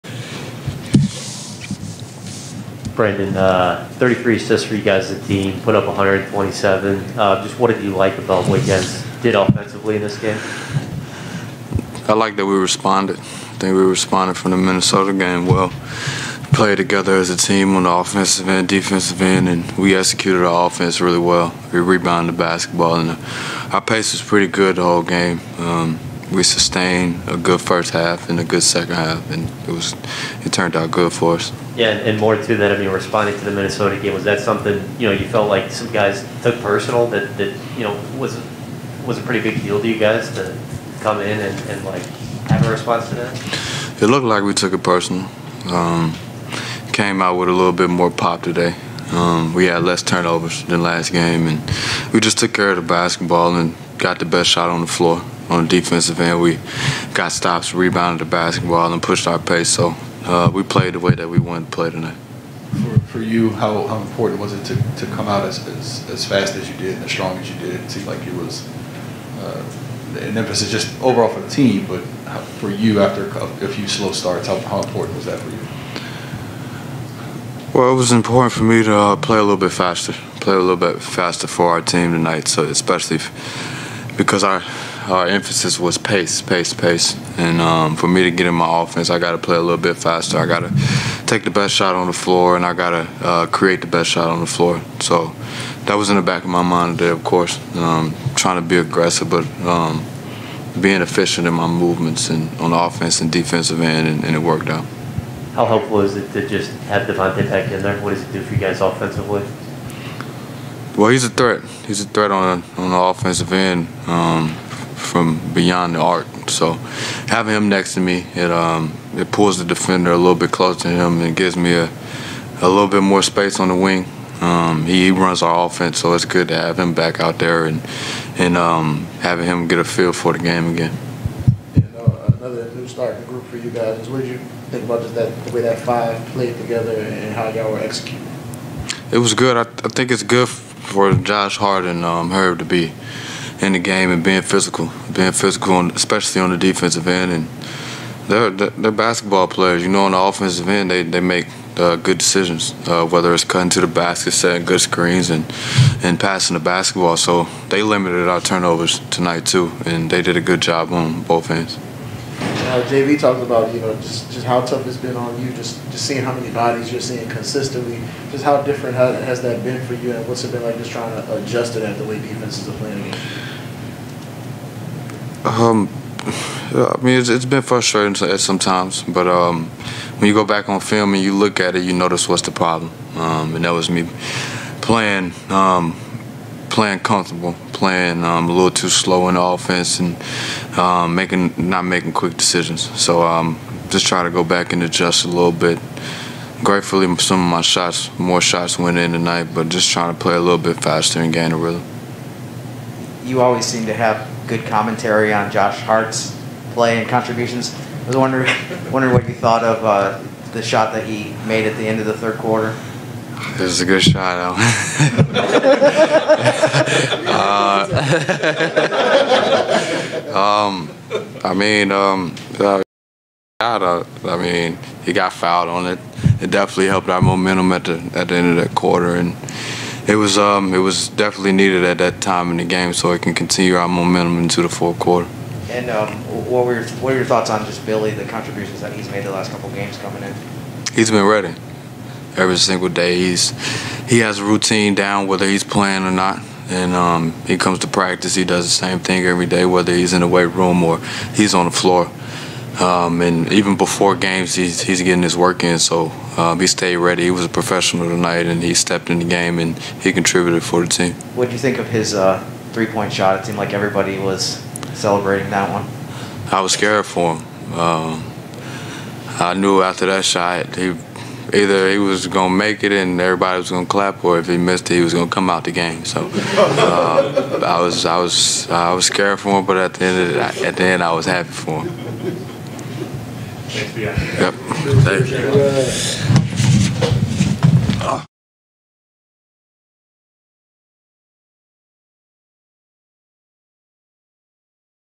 Brandon Ingram on Emphasizing Pace in Win | Pelicans-Wizards Postgame
New Orleans Pelicans forward Brandon Ingram postgame interview following the Pelicans' win vs. the Washington Wizards.